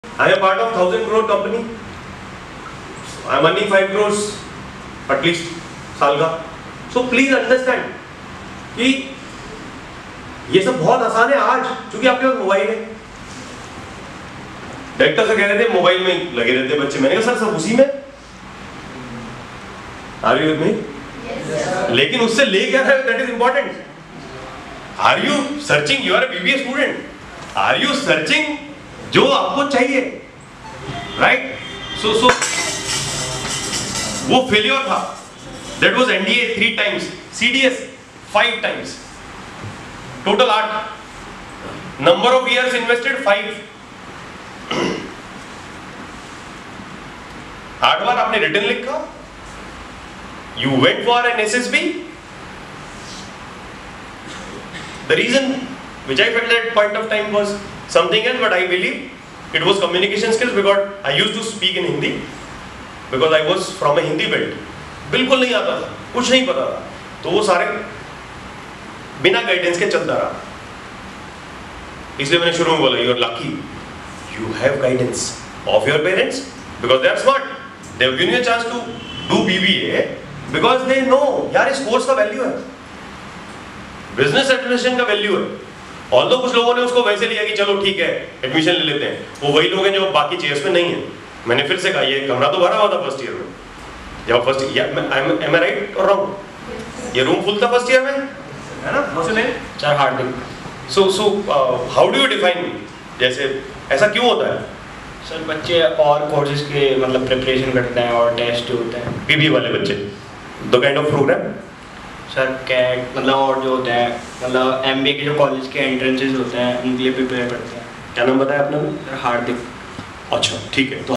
Motivational Session | Vikrant Jawla | IIC-Pt.DDUMC
Glimpses of My Story - Motivational Session by Successful Entrepreneur, Dr. Vikrant Jawla ( Director, Career Launcher )